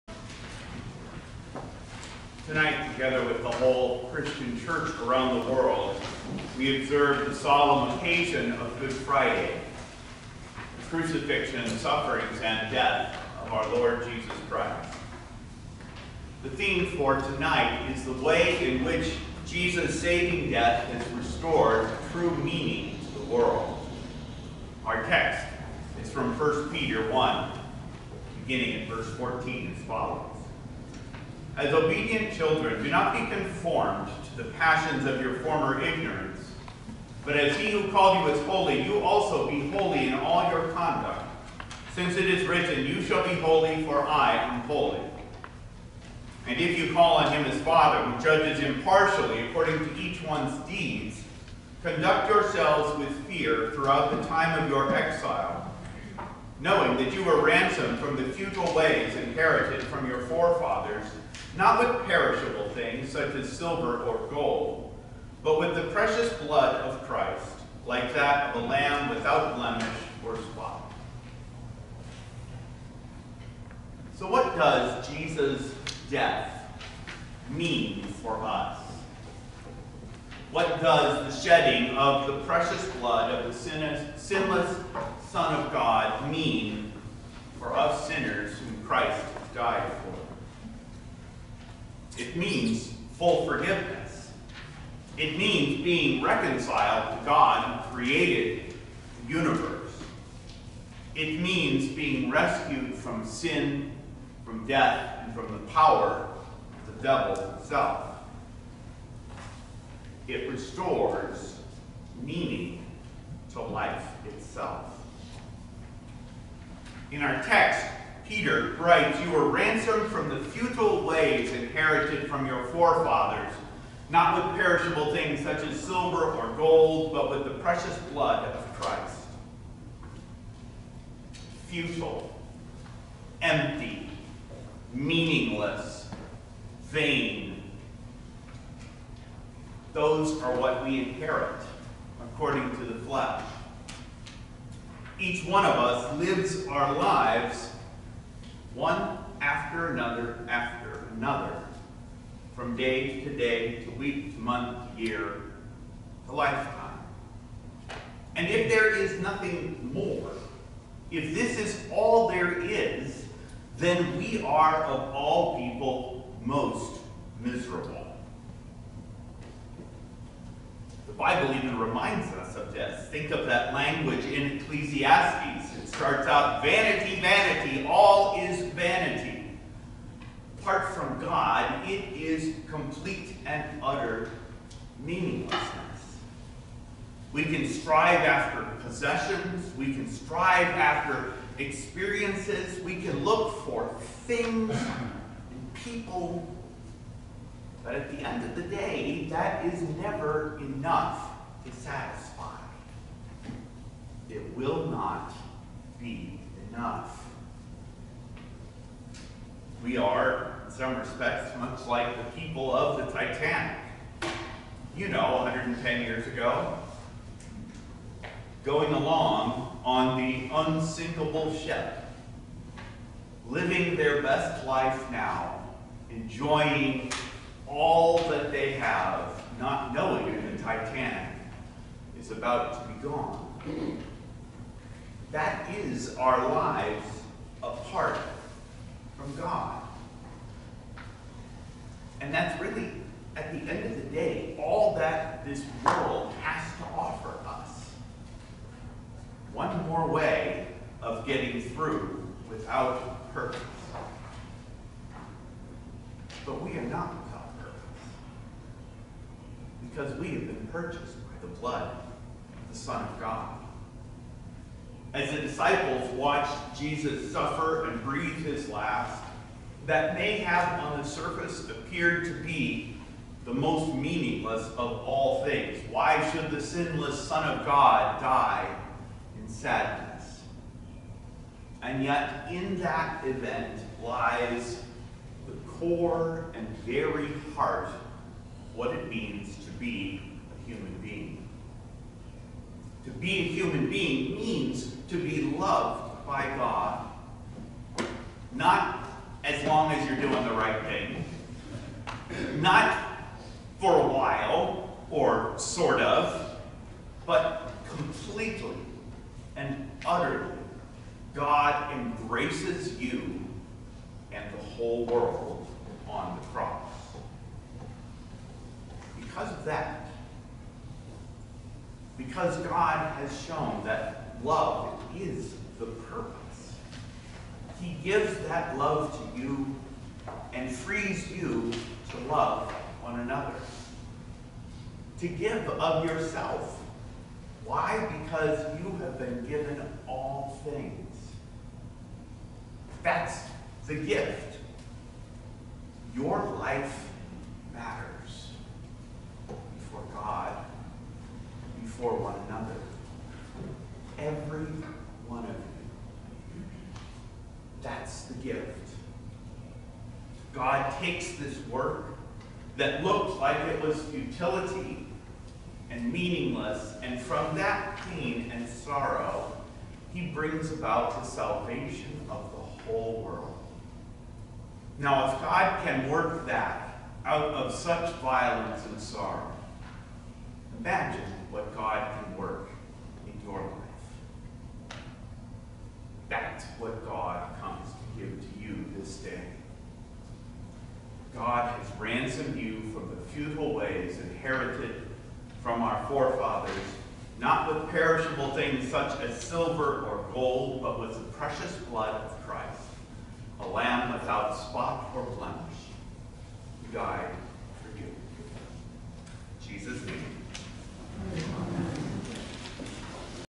HCLC - April 2, 2021 - Good Friday-Tenebrae Service - Sermon: "Jesus' Love and the Meaning of Life"
Preached at Holy Cross Evangelical Lutheran Church in Rocklin, California on Good Friday, April 2, 2021 by Pastor Todd Peperkorn.

Sermon Text: 1 Peter 1:14-19
Sermon Theme: “Jesus’ Love and the Meaning of Life”

Epistle: Hebrews 4:14–16; 5:7–9
Holy Gospel: John 18:1–19:42

The Passion

 The service of worship tonight is taken from an early Christian service called Tenebrae. The name Tenebrae is the Latin word for “darkness” or “shadows.” Tonight we will experience only a small portion of Christ’s pain and suffering the day of His crucifixion. One of the most conspicuous features of the service is the gradual extinguishing of candles until only a single candle, considered a symbol of our Lord, remains. As it gets darker and darker we can reflect on the great emotional and physical pain that was very real for Jesus that evening. Toward the end of the service, the Christ candle is hidden, typifying the apparent victory of the forces of evil over good.  At the very end, a loud noise is made (called the strepitus) symbolizing the earthquake at the time of his death (Matthew 27) and his resurrection (Matthew 28:2). The hidden candle is then restored to its place, symbolizing the triumph of good over evil. By this single light we all depart this service in silence.